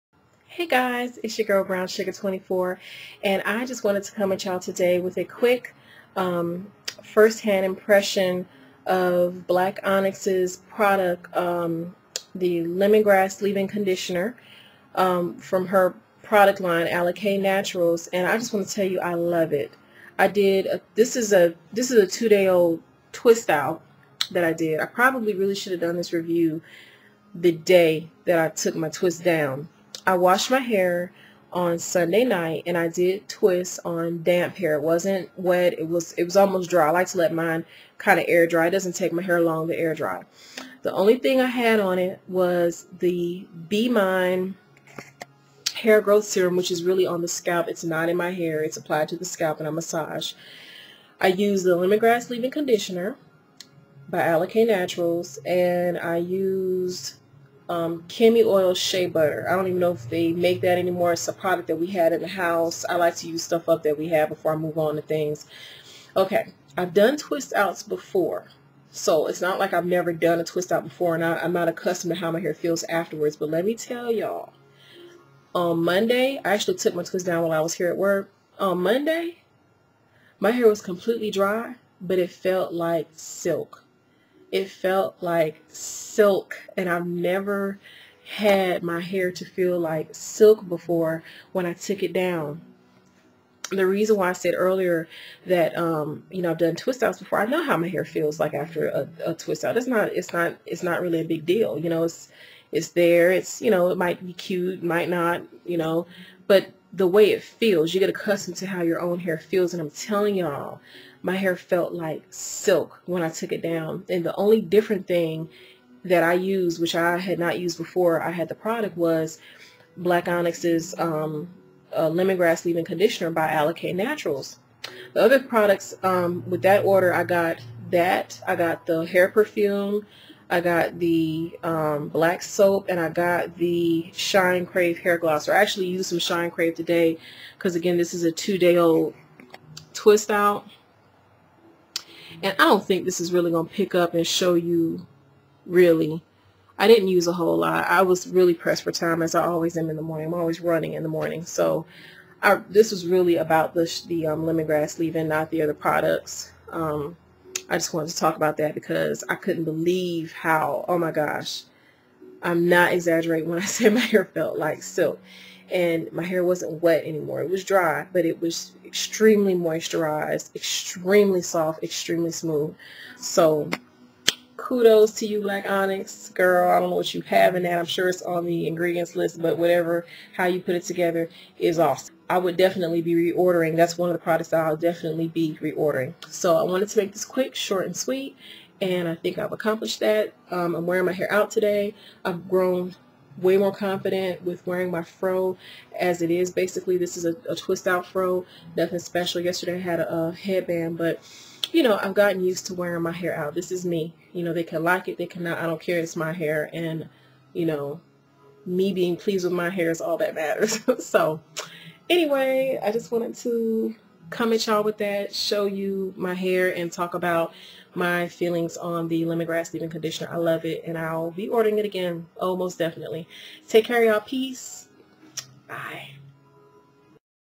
VLOG #28 - Review of Lemongrass Leave-in Conditioner by Alikay Naturals
OMG, I love this stuff! It left my hair feeling ultra soft and silky.  Will definitely purchase again!